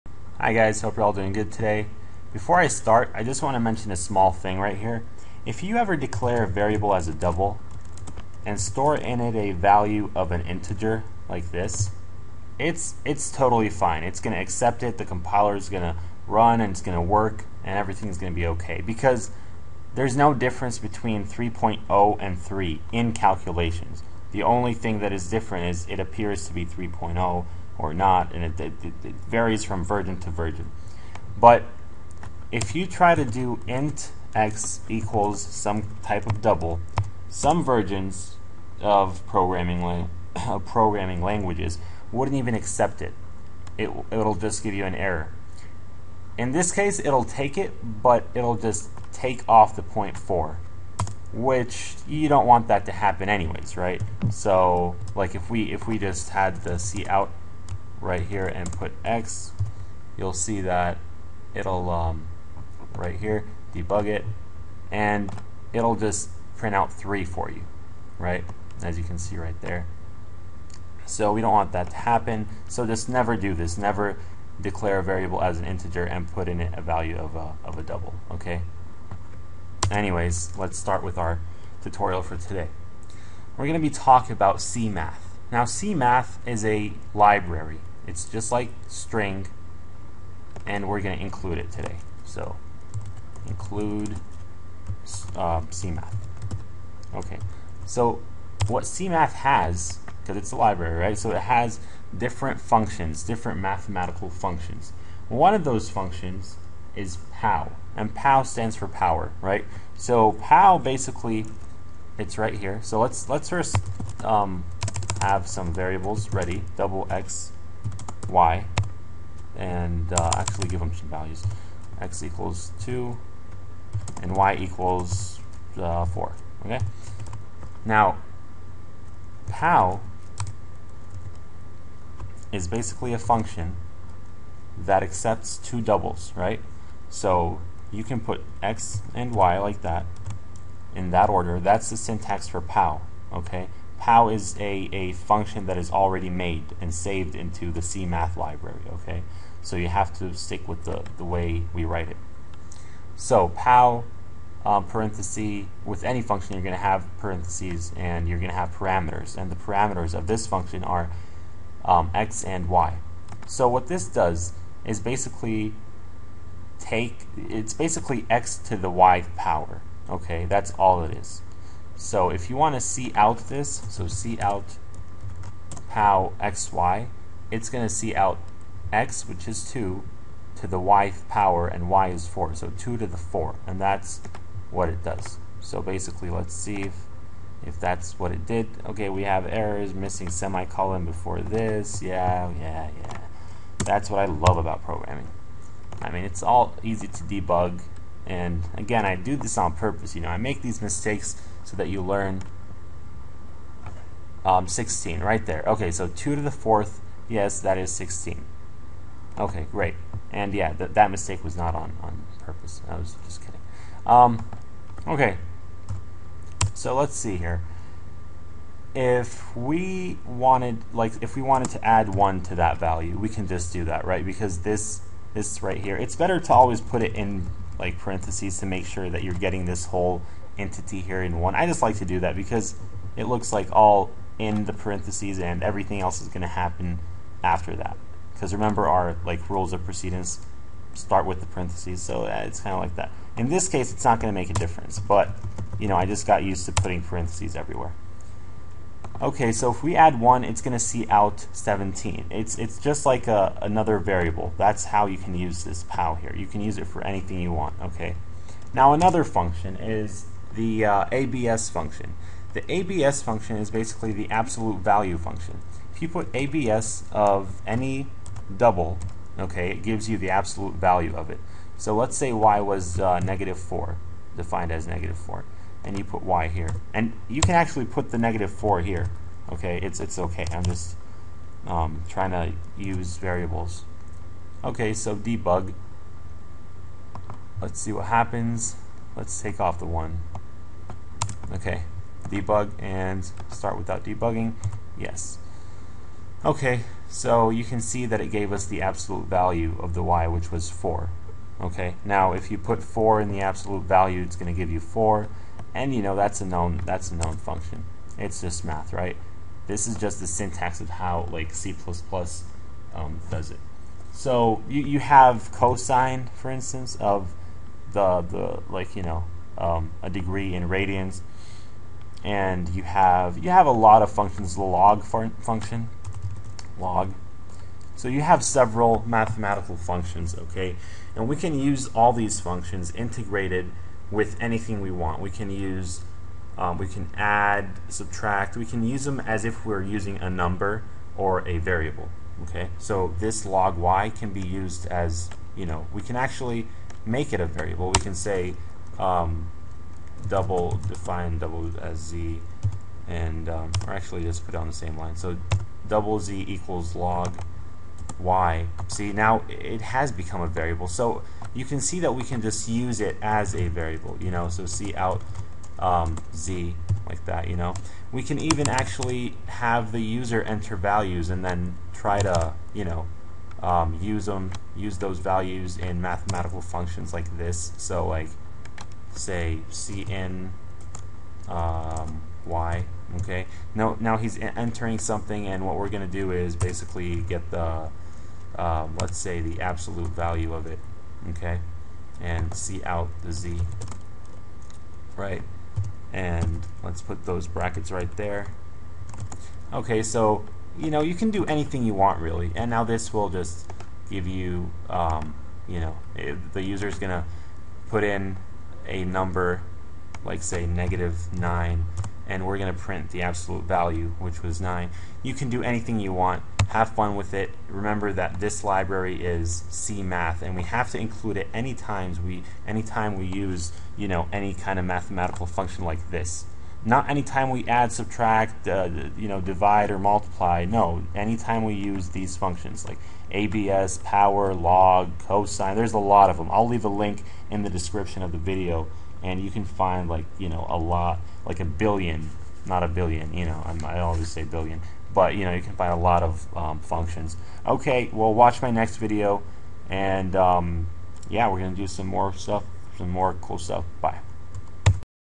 Learn programming now 15 ( Cmath library C++)
In this video I go over some of the mathematical functions that can be found in the cmath library. The video is really easy to follow and understand. If you guys have any questions, just let me know.

NOTE: The link above is not owned by me.